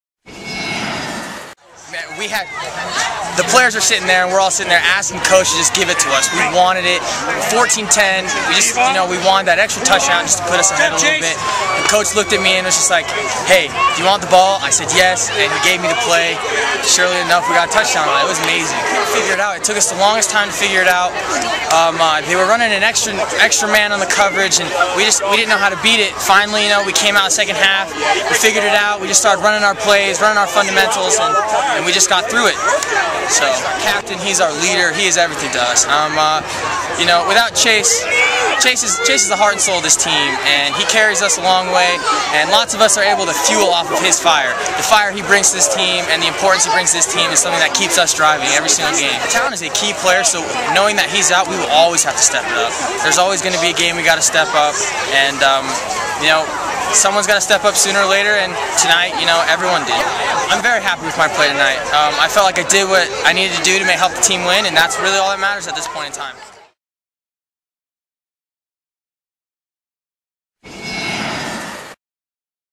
High school football: Connor Humphrey (Lone Peak Knights) post-game interview 09-01-11.
Connor Humphrey (Lone Peak Knights) post-game interview 09-01-11. After the Lone Peak Knights vs. Davis Darts Utah high school football game. The game was played at Davis High School, Kaysville, Utah.